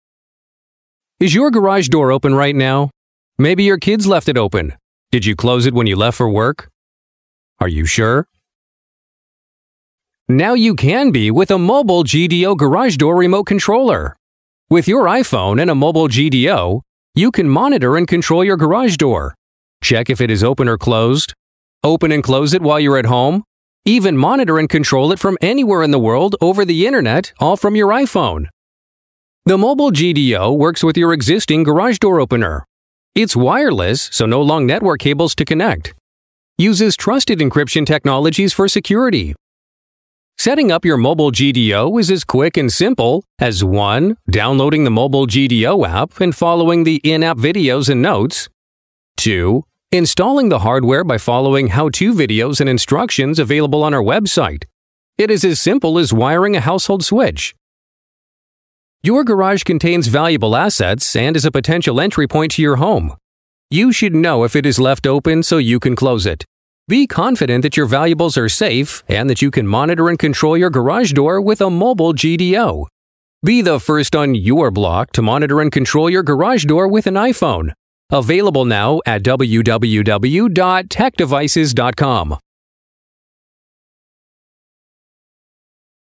Mobile GDO Garage Door Opener
The Mobile GDO device is designed to operate a garage door opener by using your Apple iPhone. The Mobile GDO connects your iPhone to the Mobile GDO garage door opener though the use of your home 802.11B/G WiFi network and the internet. The smartphone functions like a hand held garage door opener, but it can operate the garage door opener from anywhere using WiFi and internet connections. Mobile GDO also allows you to remotely check if your garage door is open or closed.

The Mobile GDO works with all garage door openers that utilize a push button switch to activate the garage door opener. Known garage door openers that work with the Mobile GDO are: Genie, Chamberlain, Liftmaster, Sears Craftsman, Raynor, Wayne-Dalton, Access Master, etc.